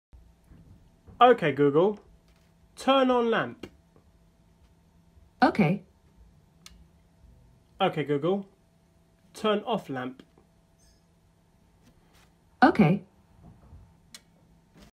MiHome and Google Home
Use your voice to control your home with the Google Home integration through IFTTT